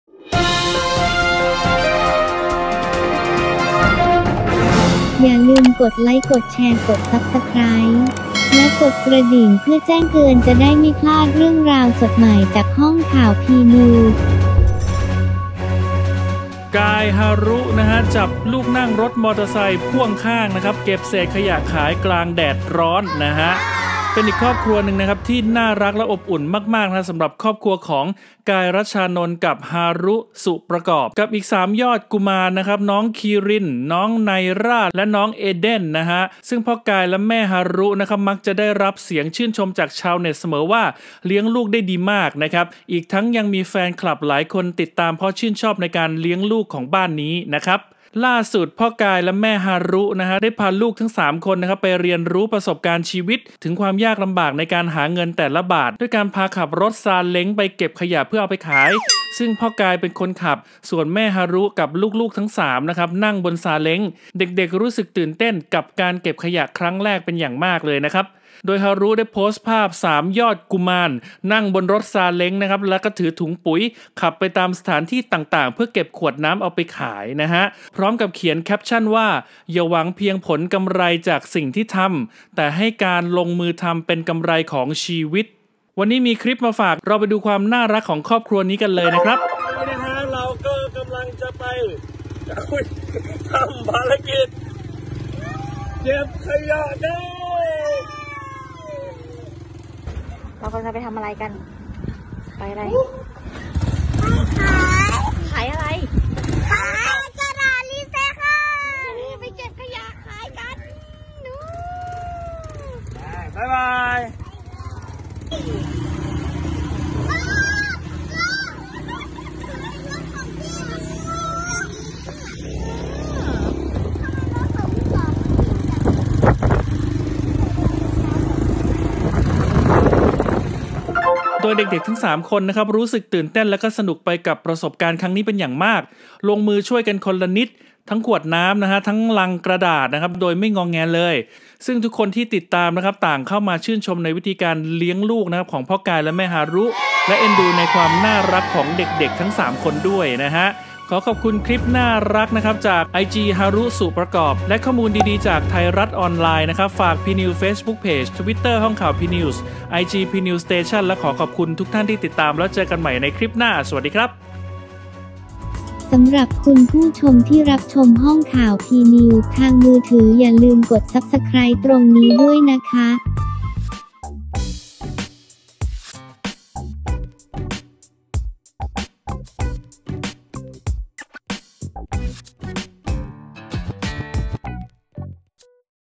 ห้องข่าวพีนิวส์ | กาย-ฮารุ จับลูกนั่งรถเครื่องพ่วงข้าง เก็บเศษขยะขายกลางแดดร้อน
ขอขอบคุณข้อมูลดีดีจาก ไทยรัฐ ออนไลน์